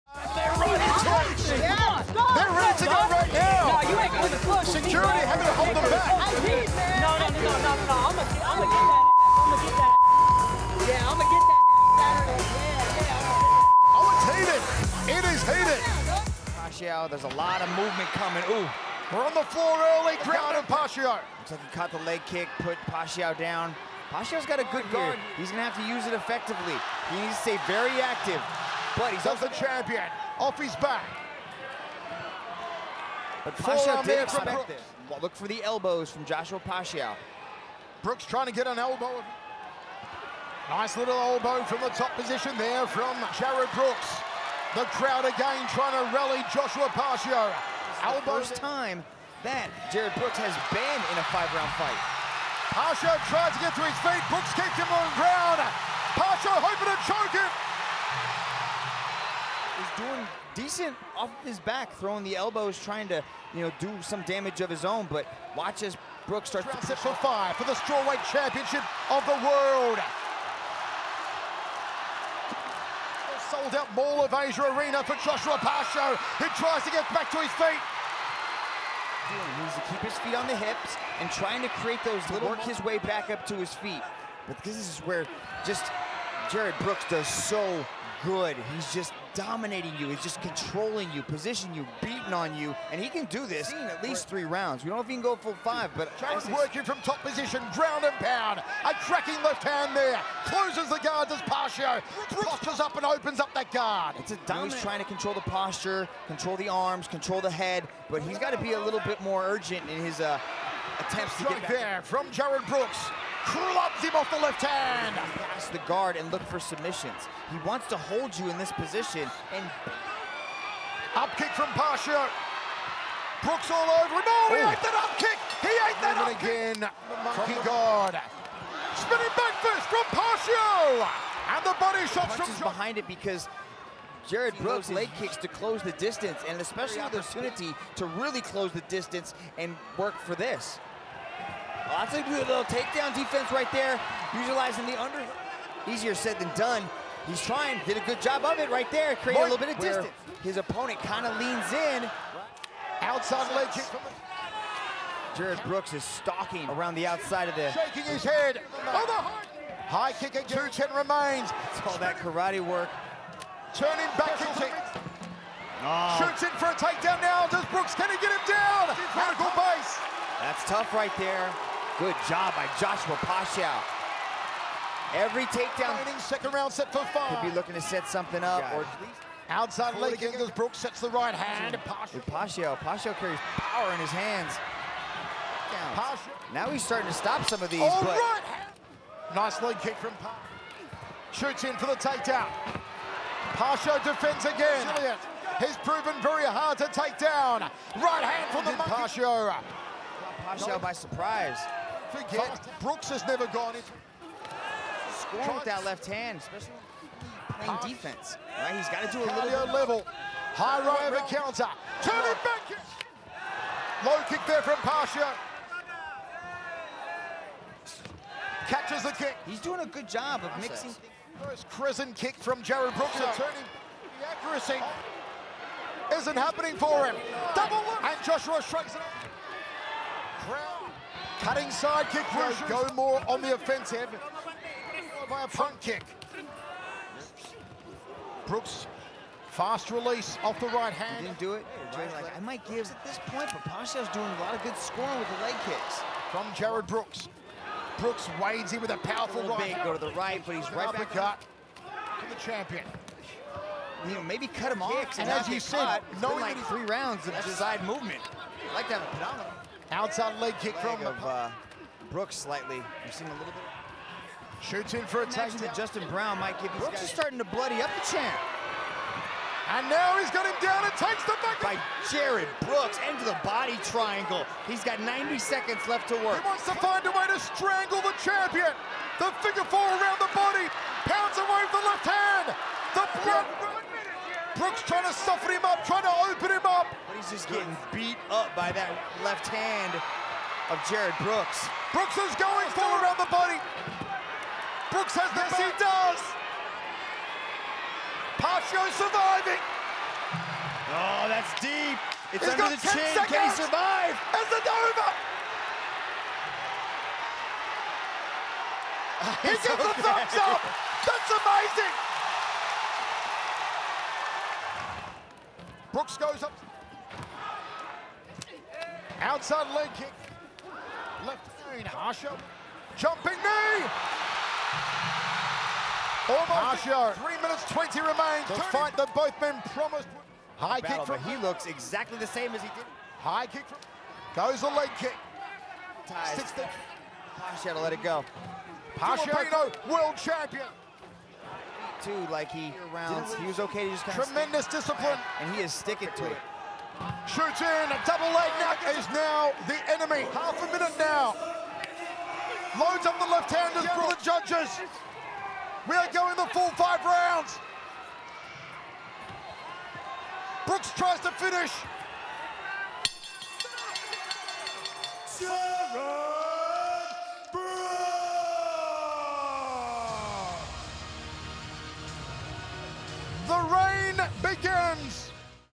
WHAT.A.WAR 😤🔥 Joshua Pacio vs. Jarred Brooks Was MADNESS
Relive all the epic action from the heated ONE Strawweight World Title collision between longtime king Joshua Pacio and American upstart Jarred Brooks in 2022!